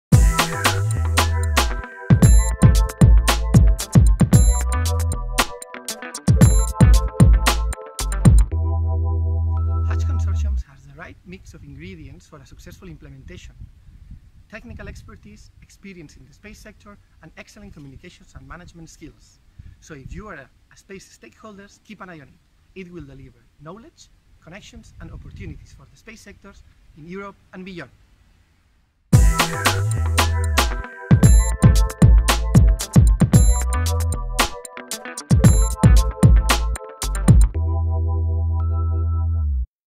Eurecat about the HATCH project
Technology Centre of Catalonia (Spain) Eurecat about the collaboration with other partners on the HATCH project.